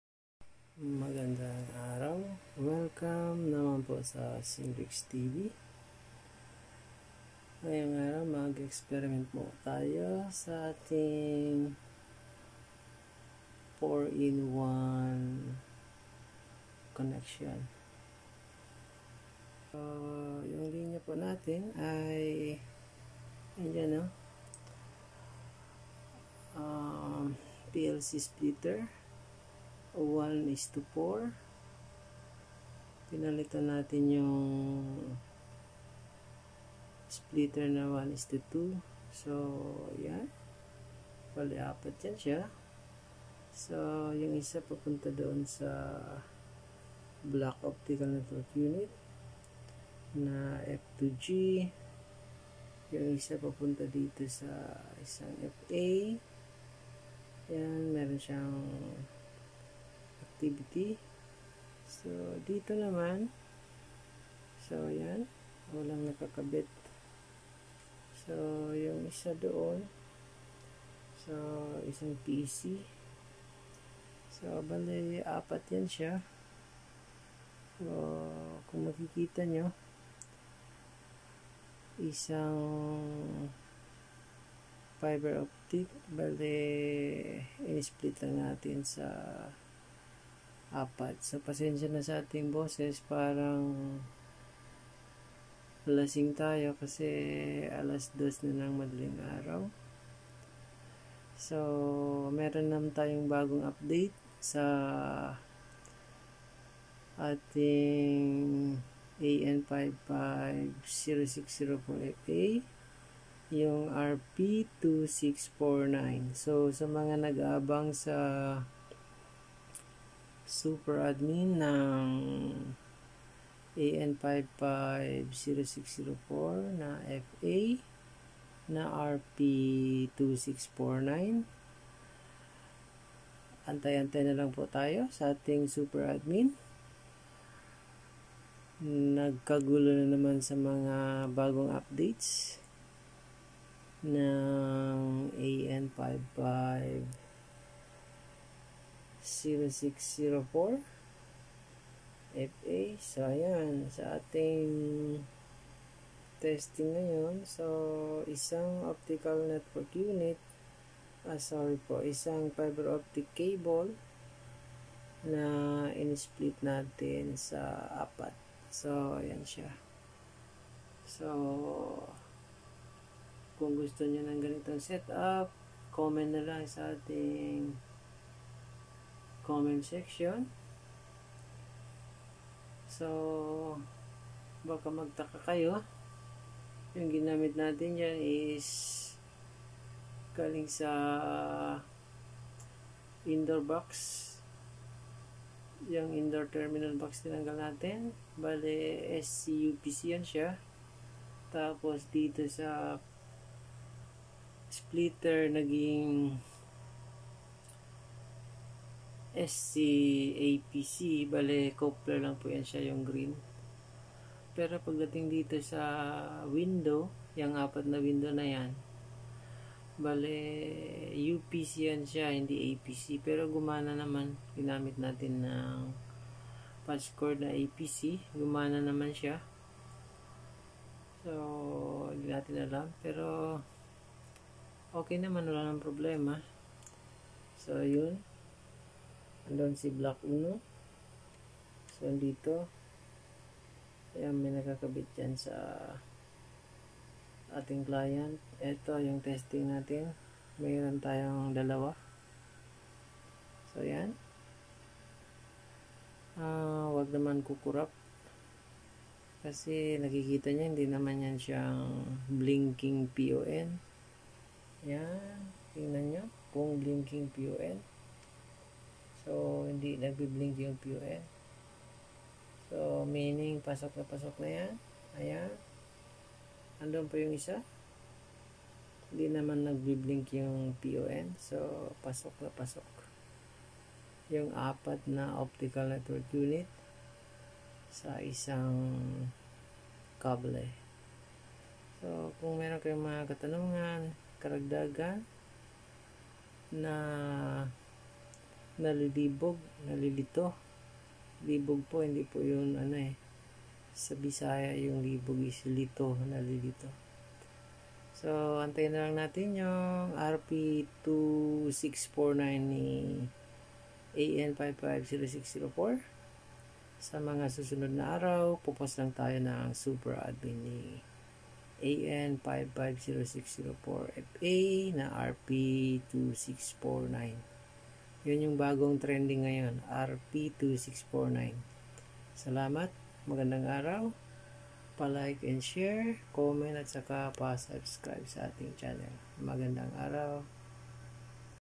SUPER ADMIN RP2649 | FOUR AN5506-04 ONE FOC
How to make connection on a single Fiber Optic Cable to four Optical Network Unit (ONU)
Super Admin An5506-04-FA RP2649